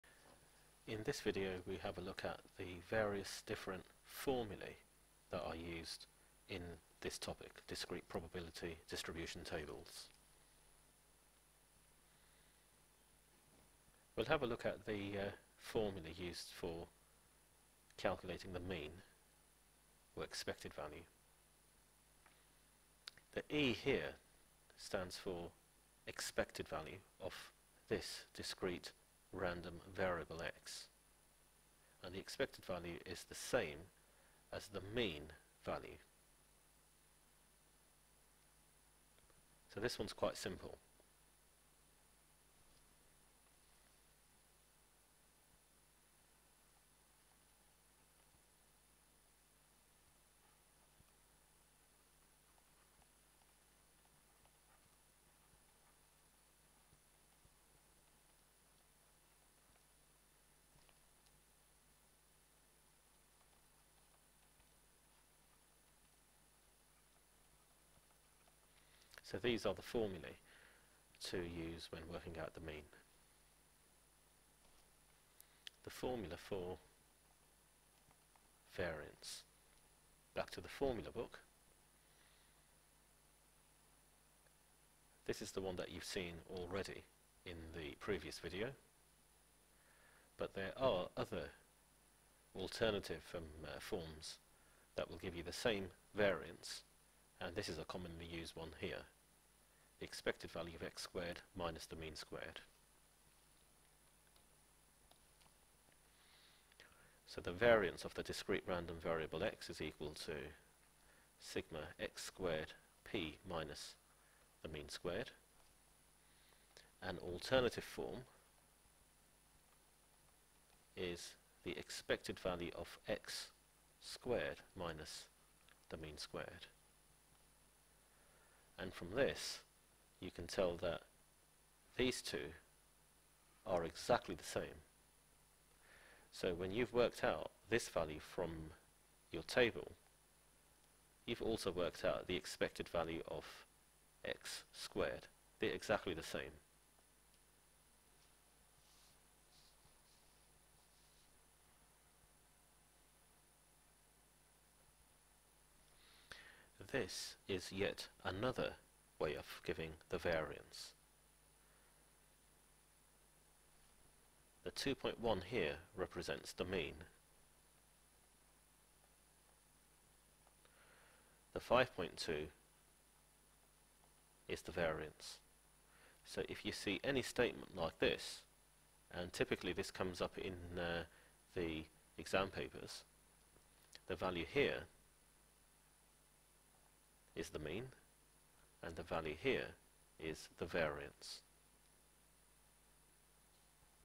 Discrete Probability Distribution Tables - Formulae
Formulae from AQA formula book.
µ = E(X) = ∑ xp
Var(X) = ∑ x²p - µ²
E(X²) = ∑ x²p